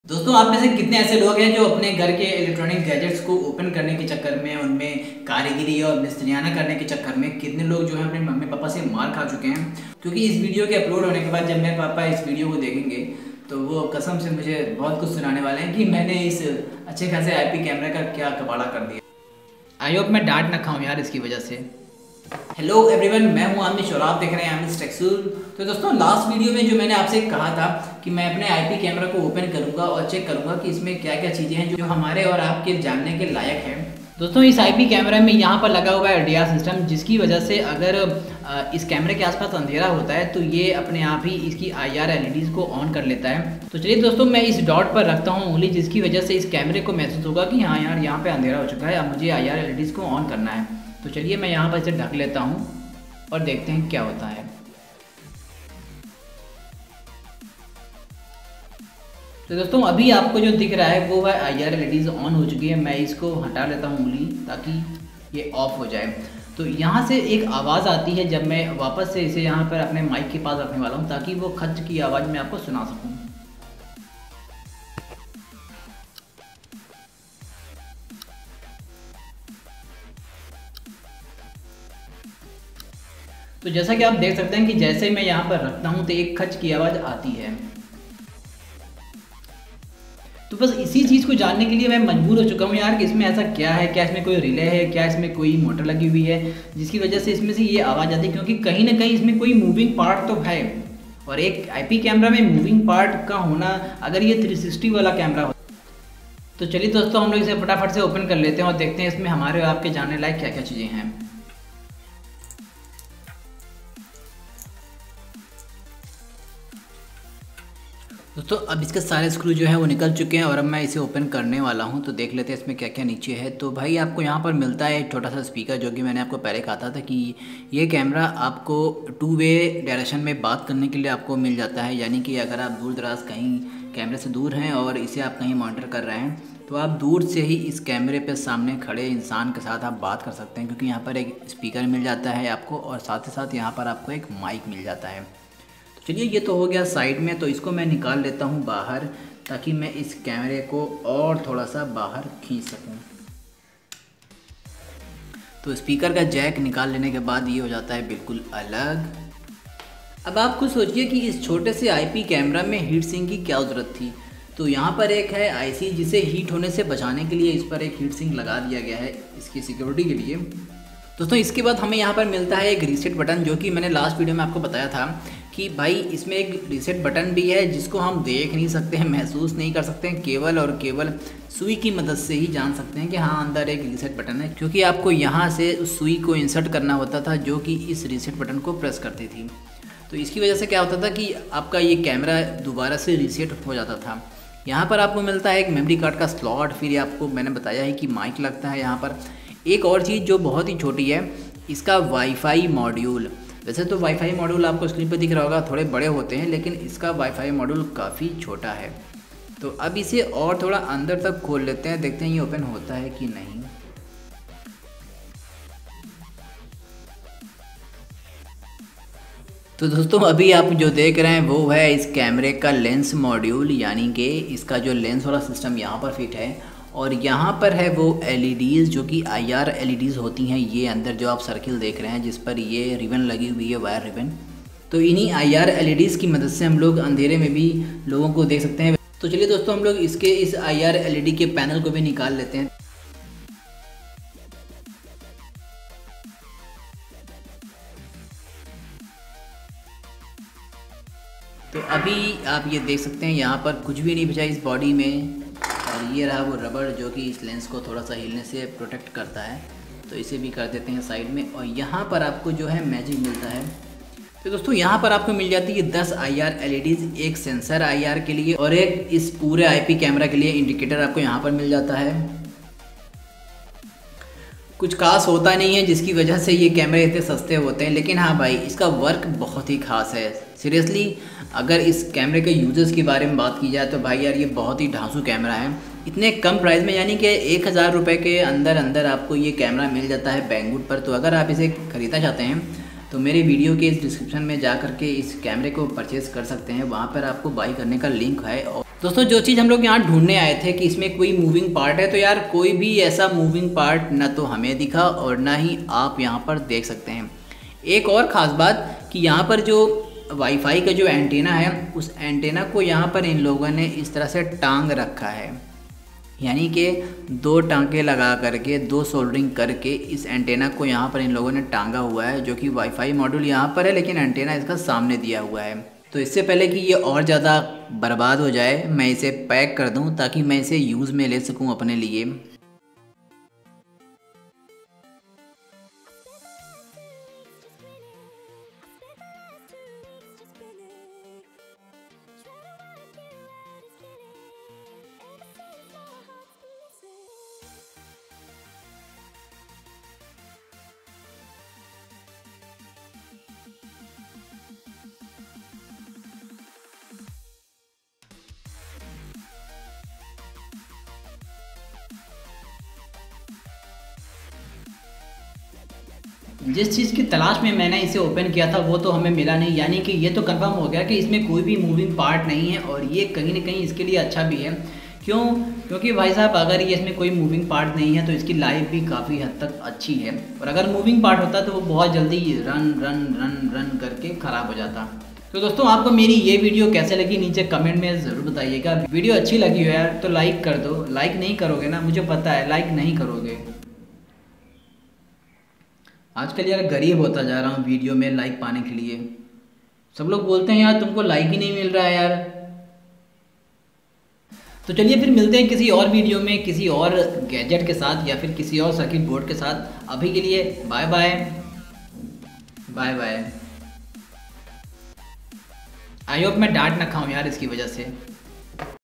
आखिर ये आवाज क्यों आती है | What's inside IP Camera and How It's Work | IP Camera Teardown
Cheapest Price Here Rs. 857

wireless ip camera price in india,cctv camera,best cctv camera brand in india,cctv camera for home,wifi camera,best budget ip camera,ip camera wireless